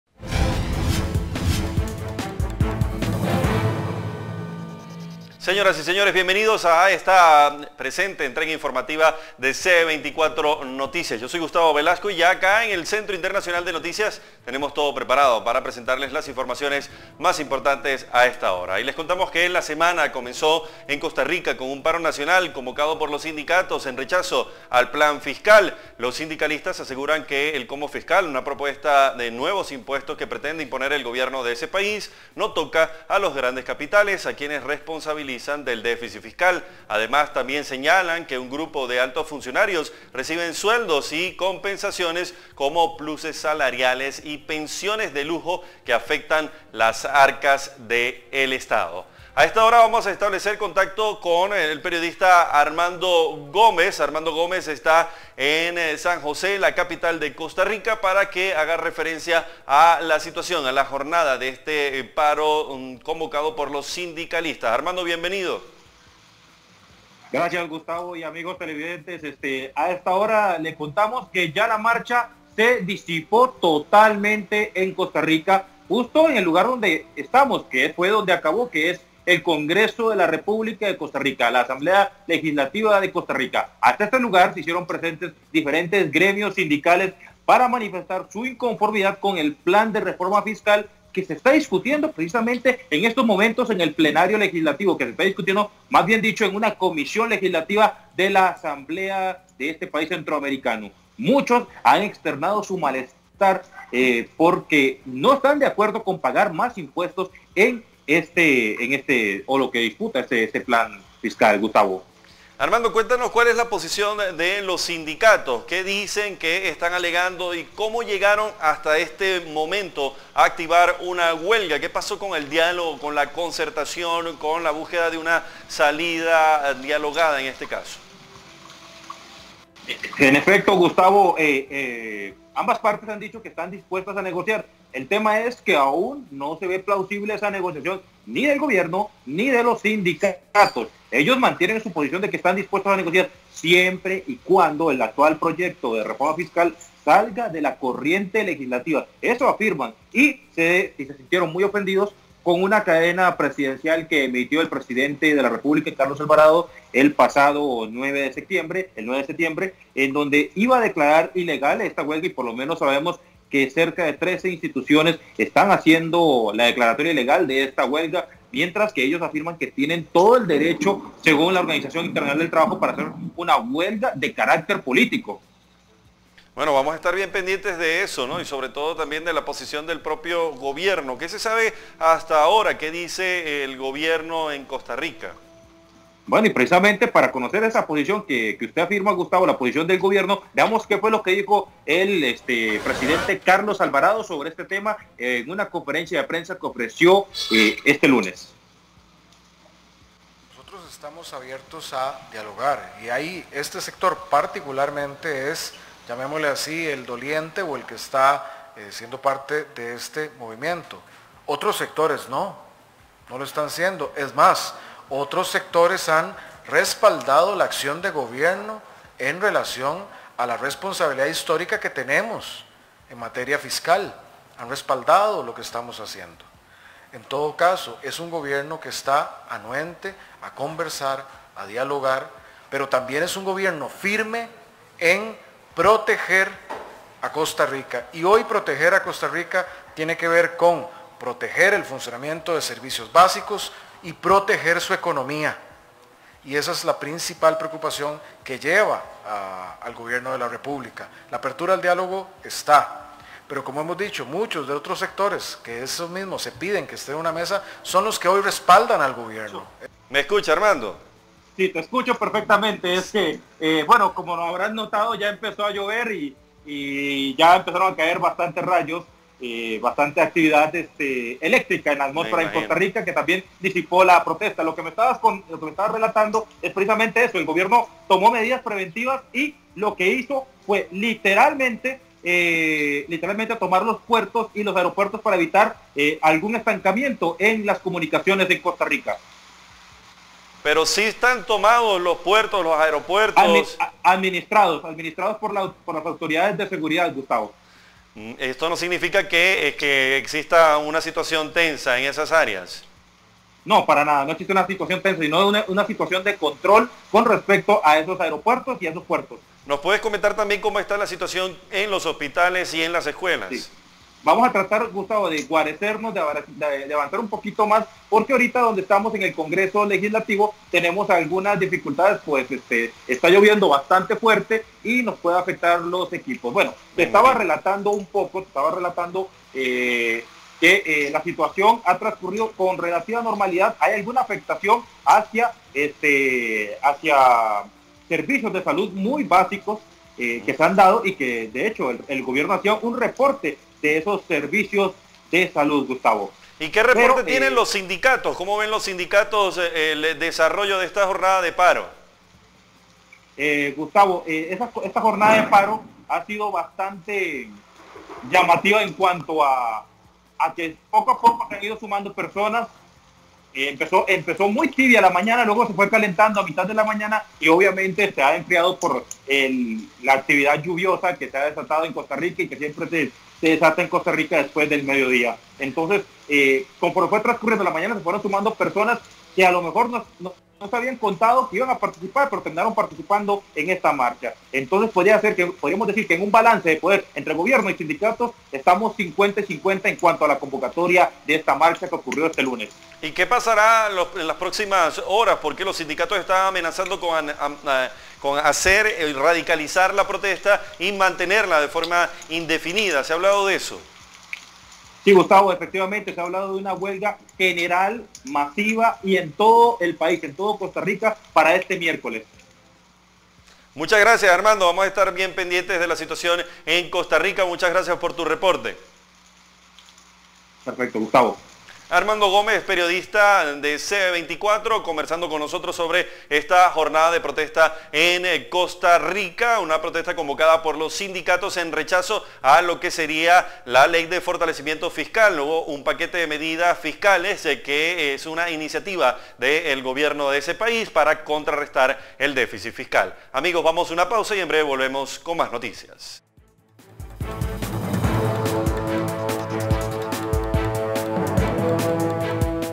Sindicatos en Costa Rica llevan a cabo huelga nacional con paro de actividades
CostaRica | Algunos servicios públicos se vieron afectados por una huelga organizada por los sindicatos en ese país centroamericano contra una reforma fiscal que se discute en el congreso. El periodista Armando Gómez está con el reporte.

24 Horas conectados con la información